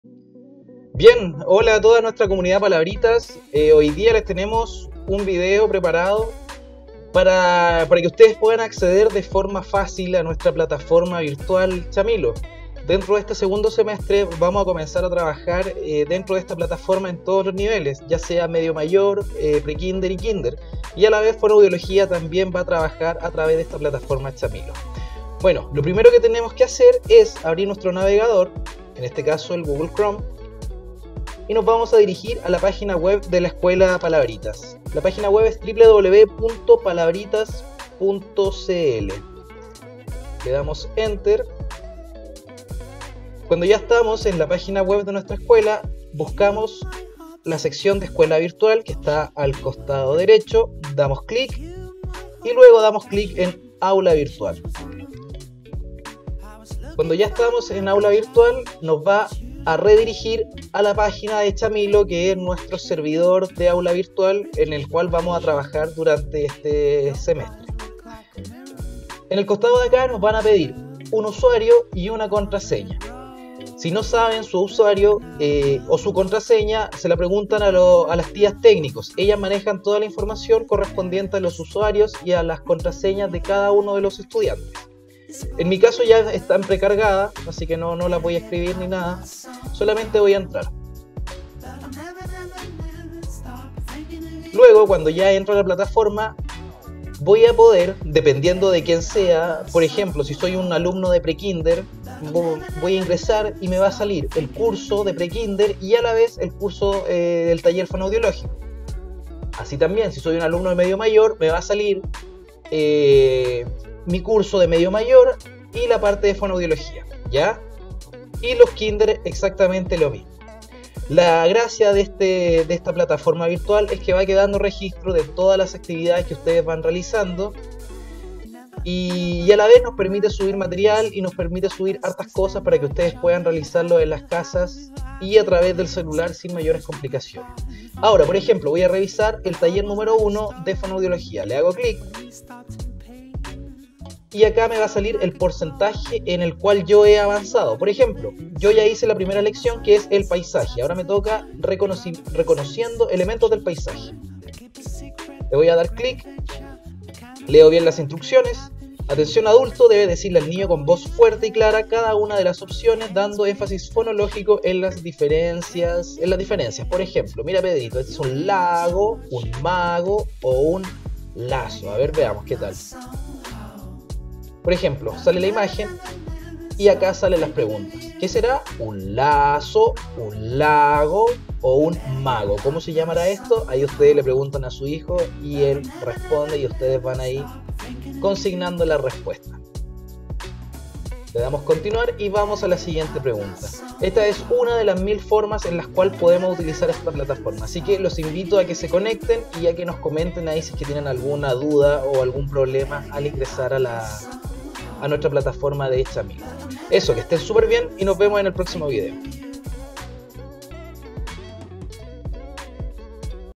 INSTRUCCIONES PARA INGRESAR A PLATAFORMA "CHAMILO"
DEJAMOS A SU DISPOSICIÓN UN BREVE VIDEO EN DONDE LES EXPLICAMOS "EL PASO A PASO" PARA INGRESAR CORRECTAMENTE A NUESTRA AULA VIRTUAL "CHAMILO".